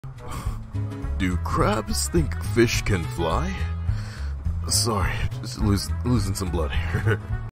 "Carb Thinking: When Fish Decide to Take Flight"
Join us on a hilarious adventure as we dive into the world of "carb thinking" and witness the extraordinary phenomenon of fish deciding to take flight. In this light-hearted video, we playfully explore the absurdity of such a notion and imagine the comical scenarios that would unfold if fish suddenly sprouted wings and soared through the sky. Get ready for a whimsical journey that will leave you chuckling and questioning the limits of imagination. Remember, in the realm of humor, anything is possible!

Regenerate response